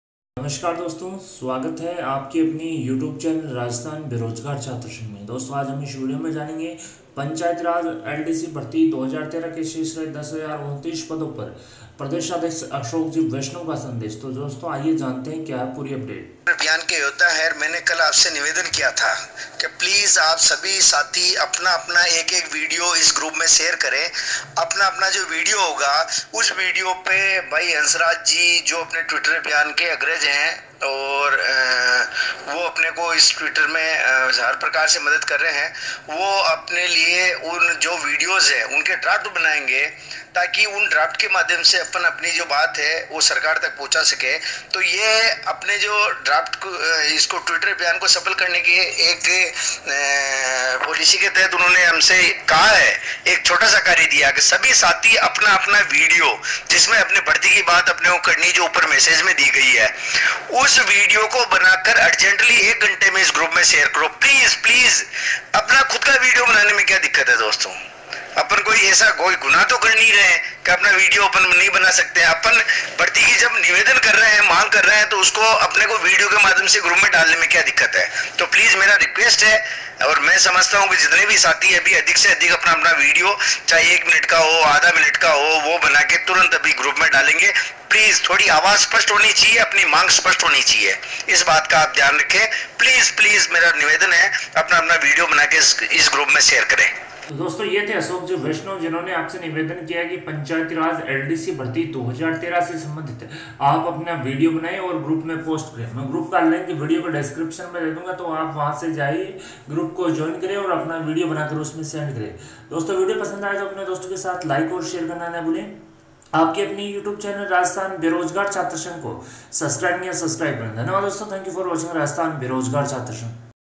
Praj Dept एलडीसी भर्ती-2013 के 10,029 पदो पर नियुक्ति को लेकर Ashok Veshnav का Massage 11 July, 2020
Praj Dept एलडीसी भर्ती-2013 के 10,029 पदो पर नियुक्ति को लेकर Ashok Veshnav का Massage